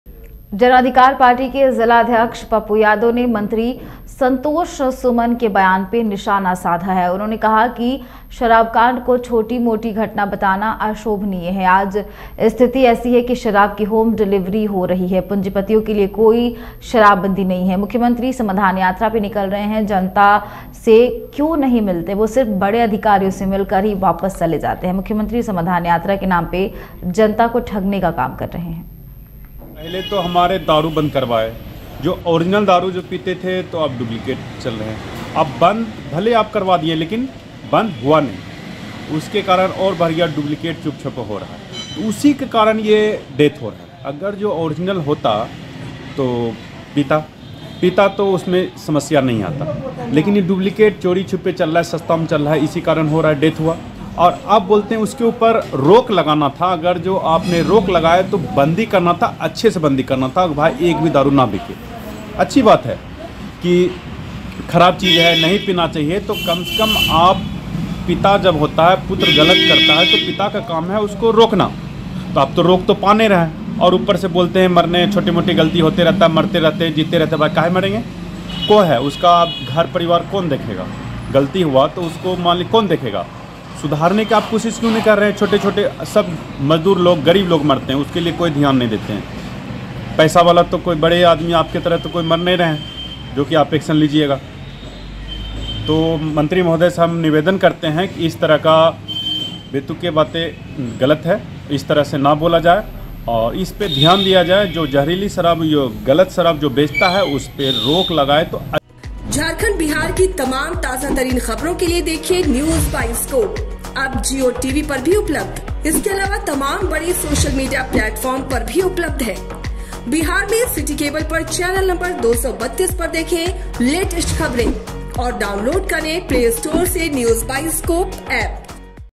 जन अधिकार पार्टी के जिला अध्यक्ष पप्पू यादव ने मंत्री संतोष सुमन के बयान पर साधा निशाना
District President of Jan Adhikar Party Pappu Yadav targeted the statement of Minister Santosh Suman

22Scope Live TV | 22 Scope | Hindi News | 22 Scope लाइव